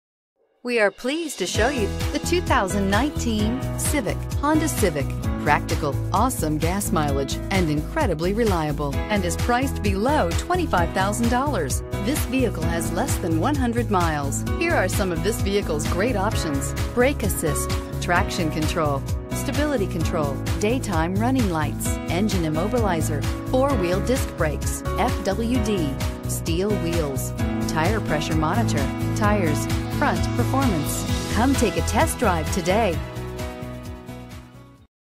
2019 Honda Civic Sedan 39763
2019 HONDA CIVIC SEDAN Mesa, AZ
Stock# 39763

6229 E Auto Park Dr

Don't miss this 2019 Honda Civic Sedan. It's equipped with Variable transmission and features a AEGEAN BLUE MET exterior. With 8 miles, you'll want to take this car home. Make a great choice today.